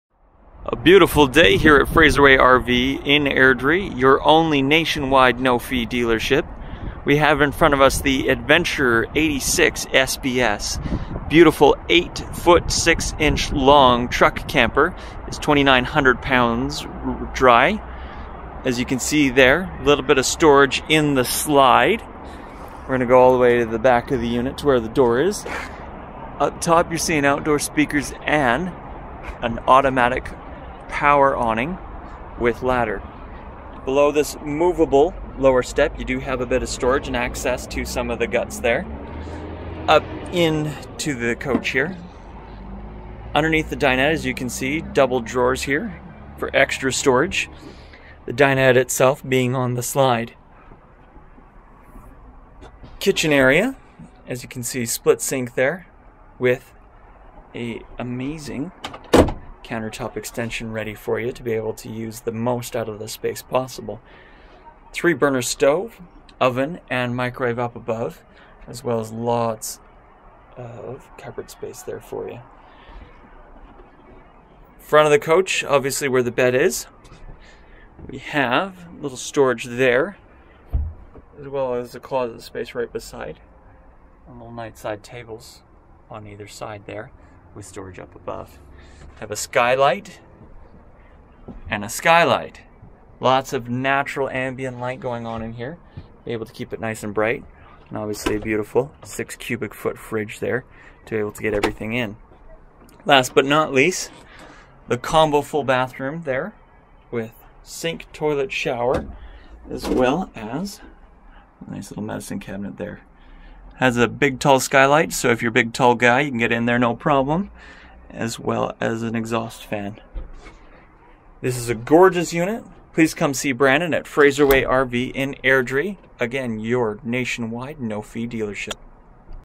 2014 Adventurer 86SBS
Brief Walkthrough of the new 2014 Adventurer 86SBS. For further details see Brandon @Fraserway RV in Airdrie.